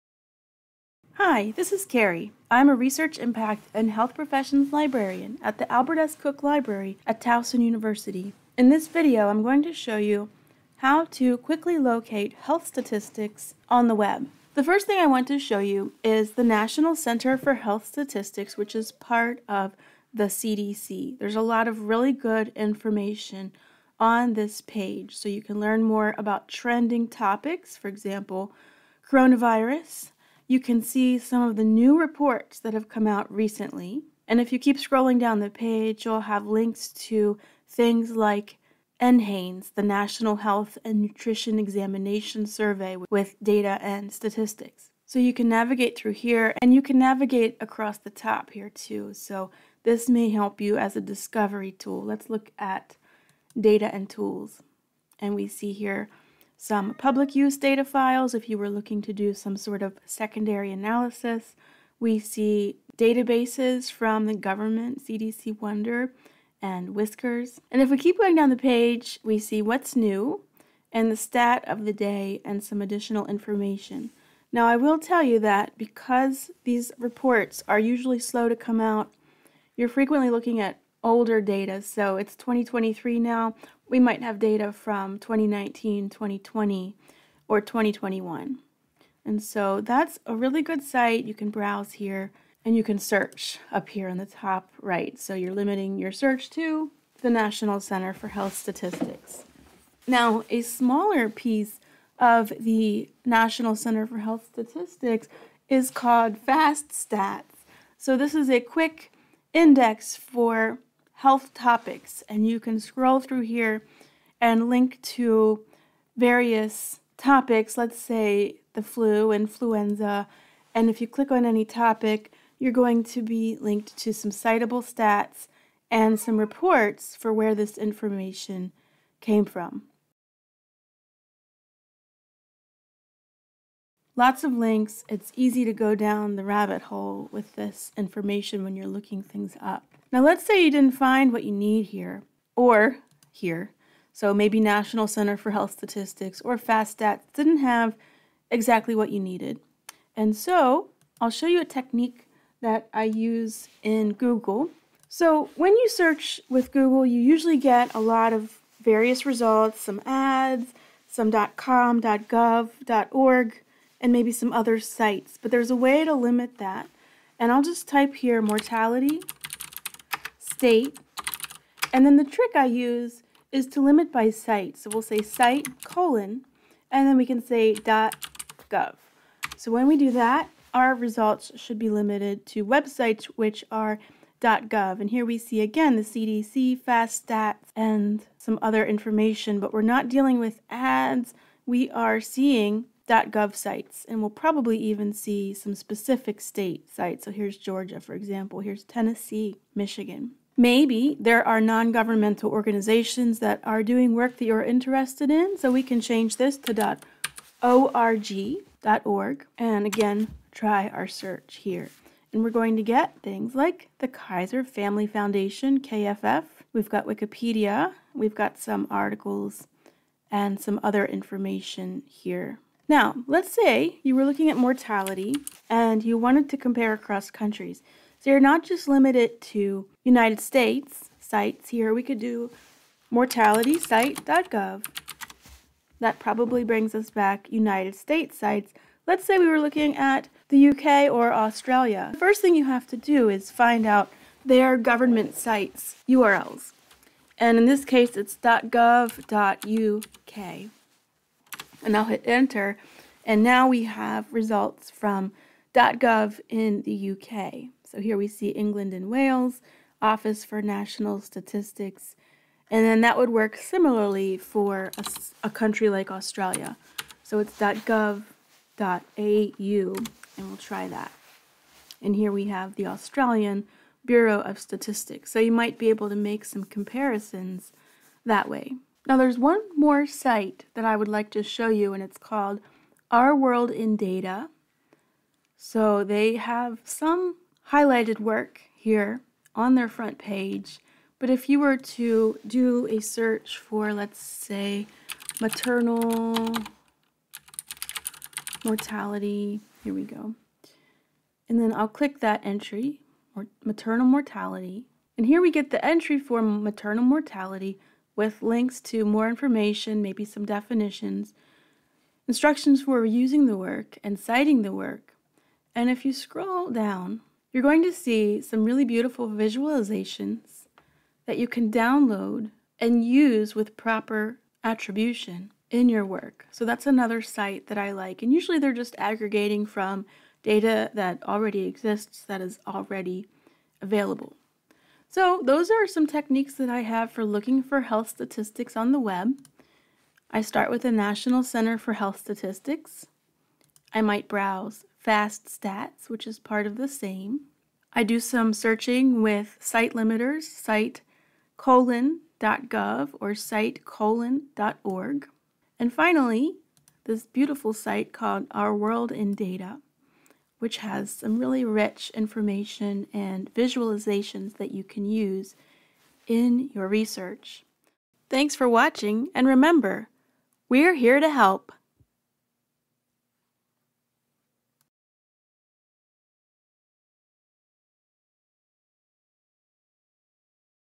Find Health Statistics on the Web
In this video, we show you how to find health statistics on the web using a few quick techniques and free websites.

0:22 National Center for Health Statistics from the CDC
1:49 FastStats from the CDC
2:38 Site Specific Searching
4:45 Our World in Data